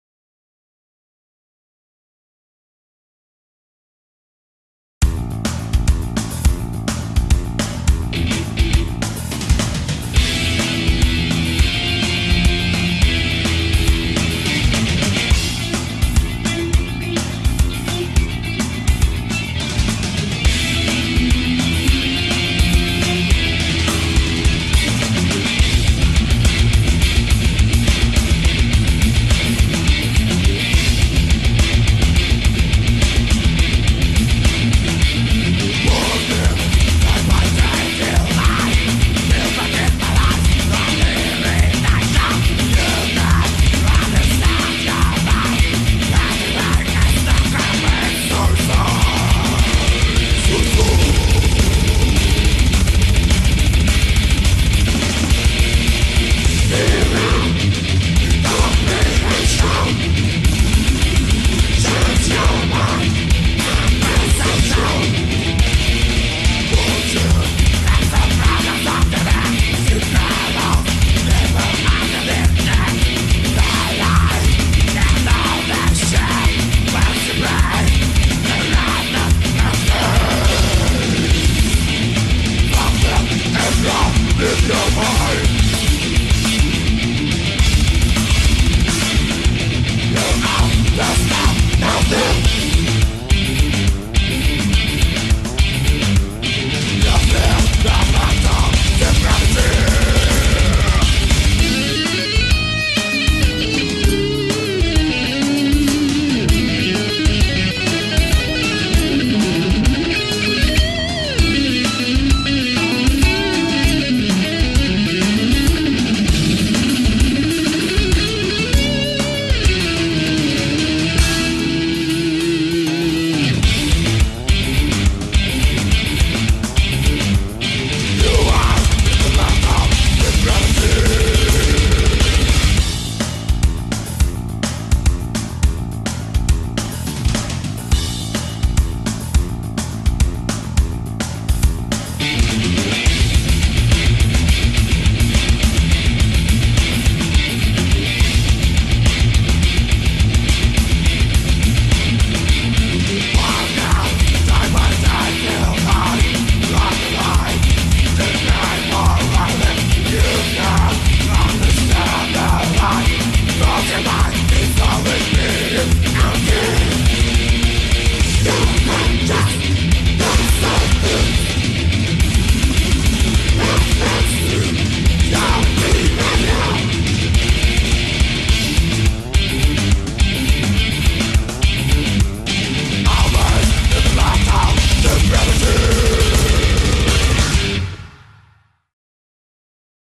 Mental Depravity
Home made videoclip of russian progressive metal band Mental Depravity.